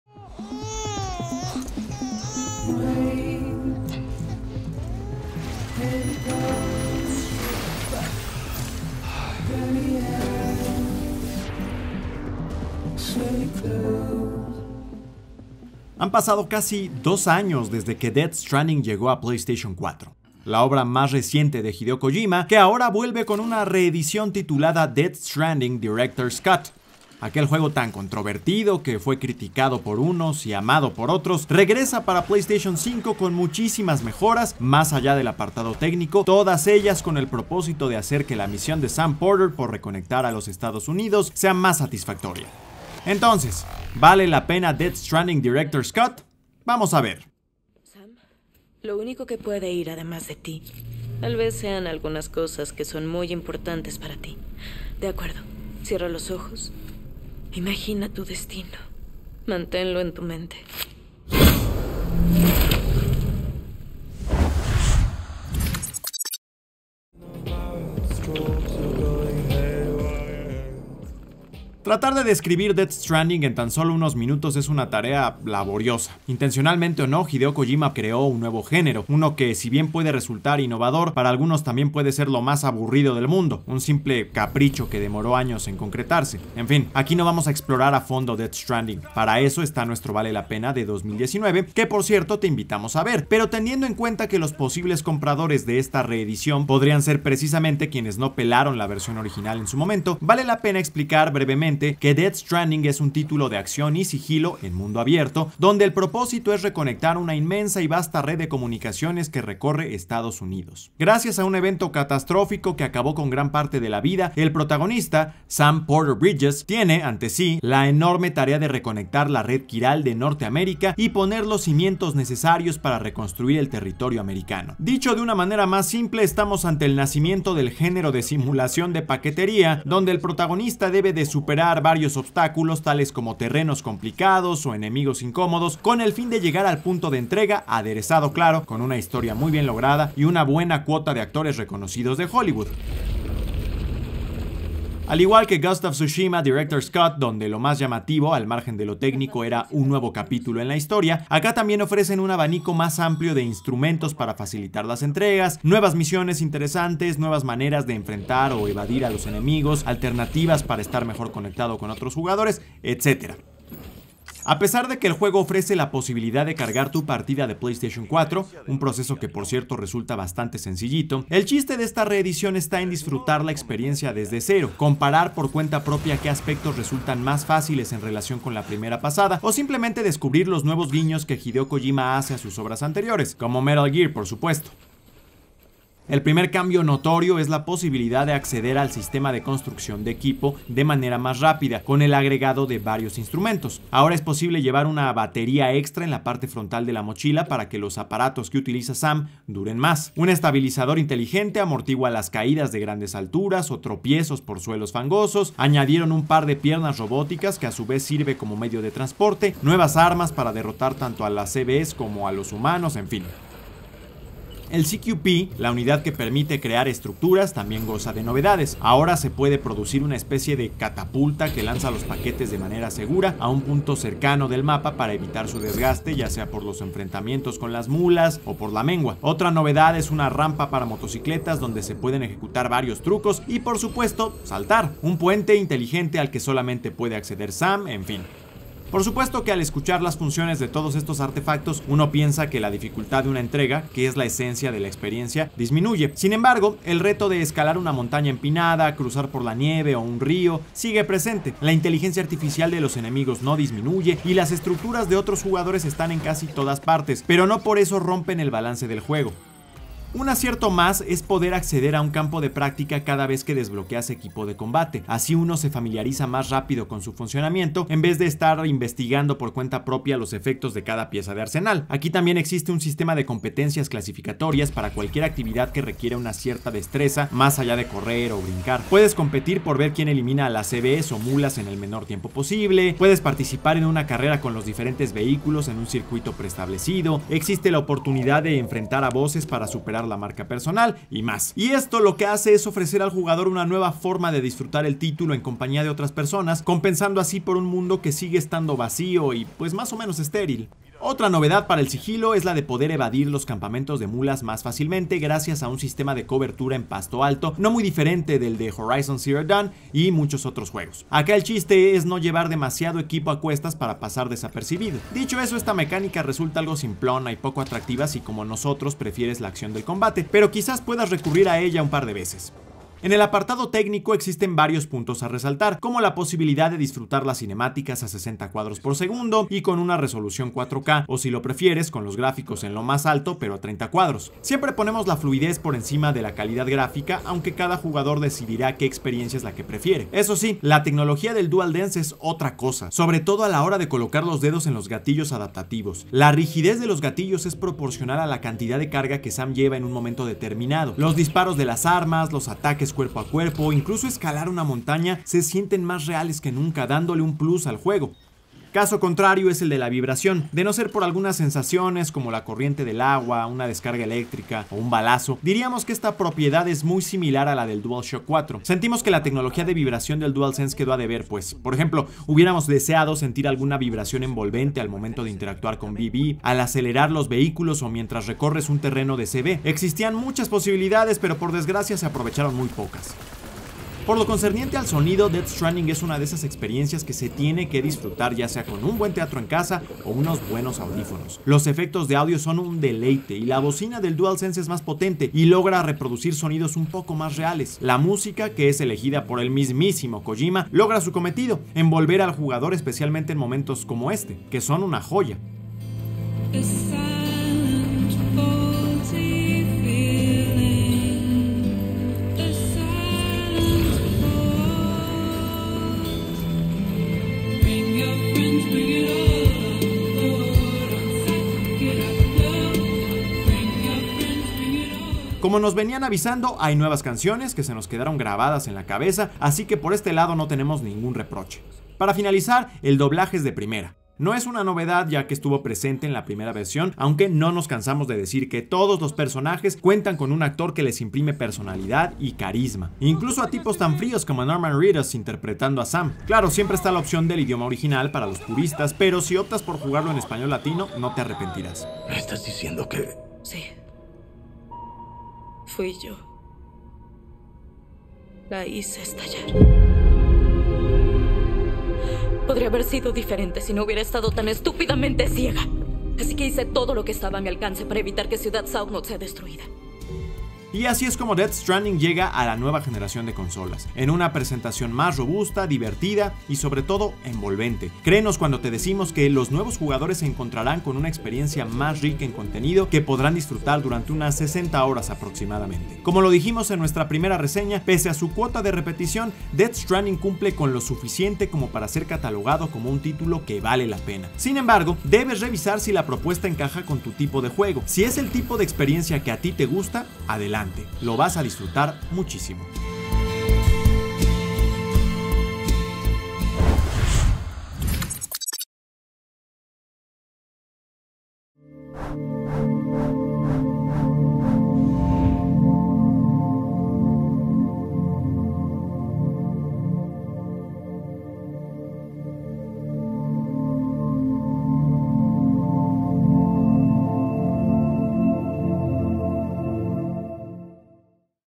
Death Stranding Director's Cut: ¿Vale la pena?
Después de 2 años de su estreno original, Death Stranding regresa ahora bajo el formato Director's Cut para PlayStation 5. Con nuevos elementos que aportan más variedad a la jugabilidad, nuevas misiones y mejoras en el apartado técnico, ¿vale la pena darle una oportunidad a la obra de Hideo Kojima?